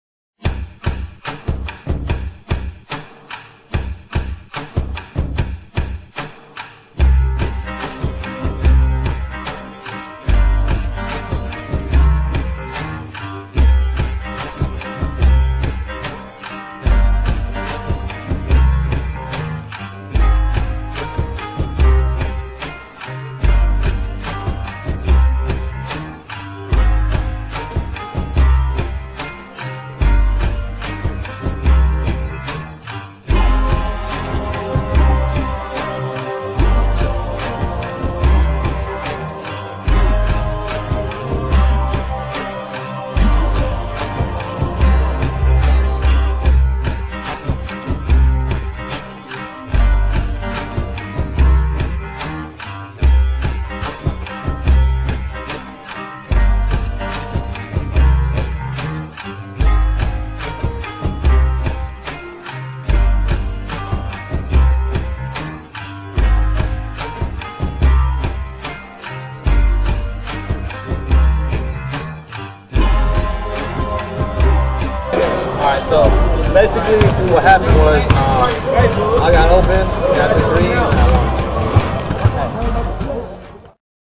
Uhd Highlights
JUST SOME FEW SHOTS JUGO TV CAPTURED LIVE AND  DIRECT IN UNIVERSITY OF HOUSTON BBALL COURT